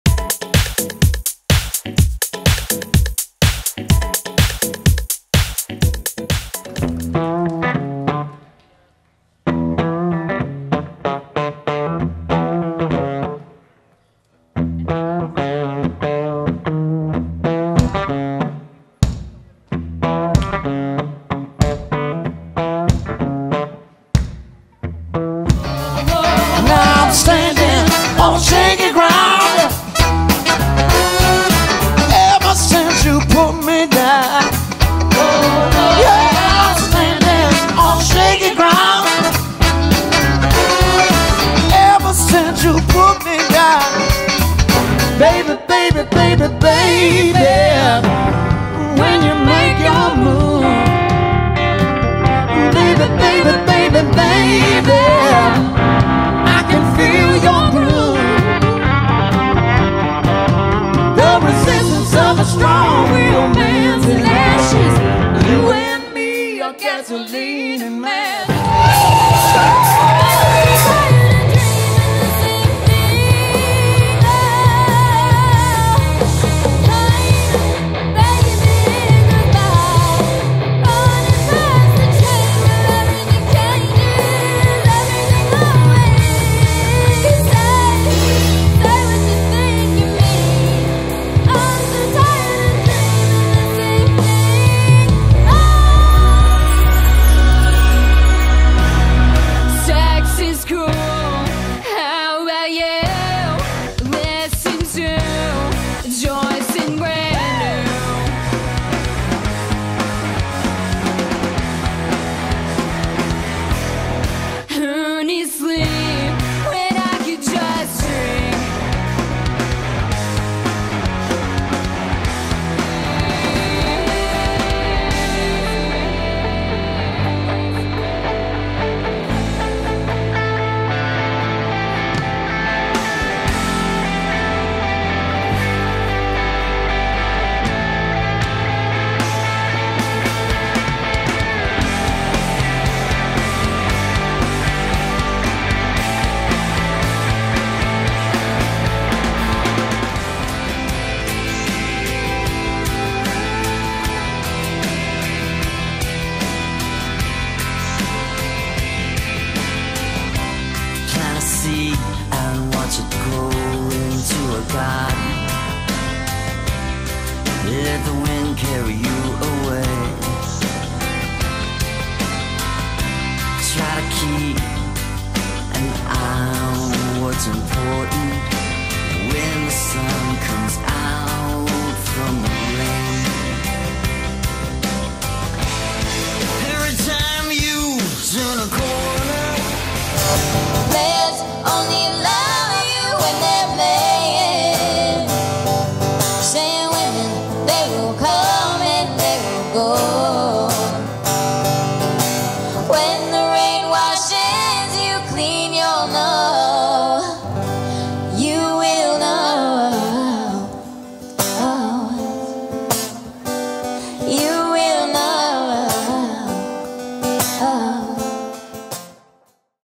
Wholly Post Sizzle Reels: EPKs for Bands
Here's a compilation of various EPKs (Electronic Press Kits) that I've done for various musicians and bands.